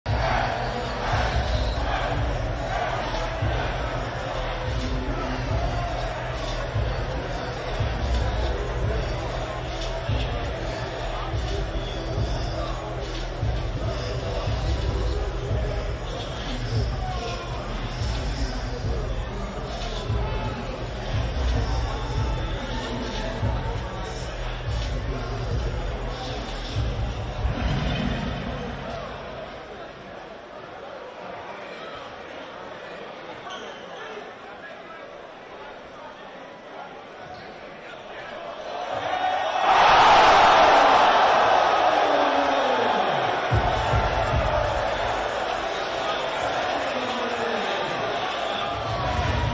LeBron James Vibing at Drew League Then Throws Down HUGE Poster
This man LeBron is living his best life.